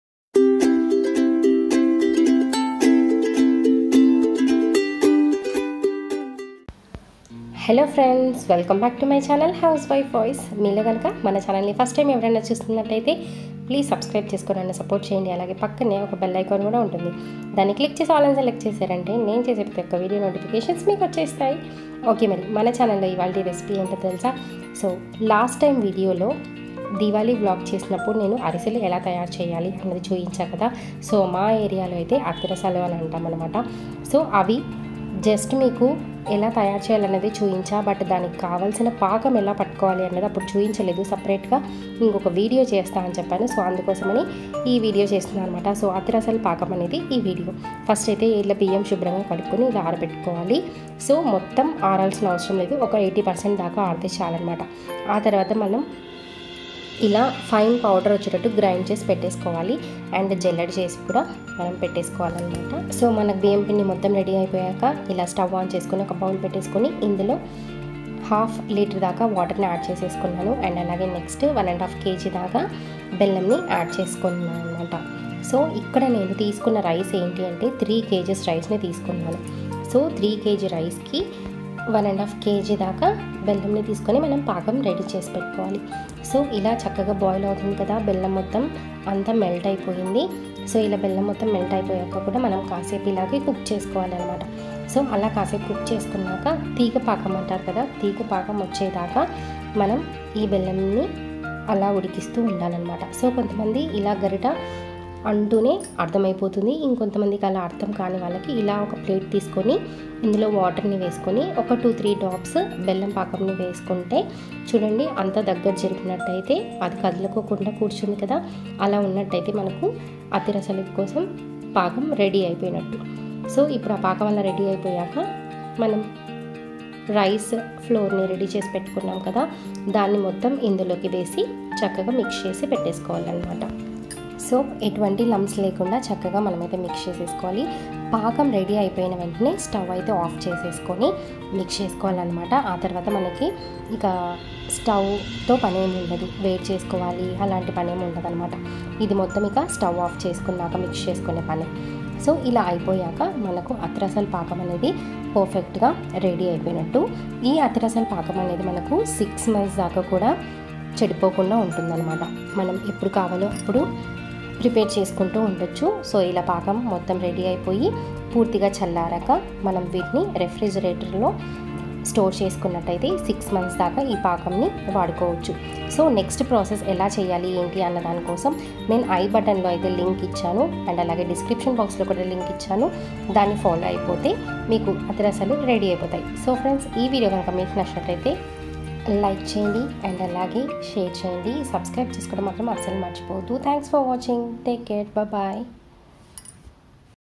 ఇలా అరిసెల పాకం చేసి చూడండి అరిసెలు పర్ఫెక్ట్ గా వస్తాయి| Ariselu Recipe|Athirasalu Pakam 👍
Thankyou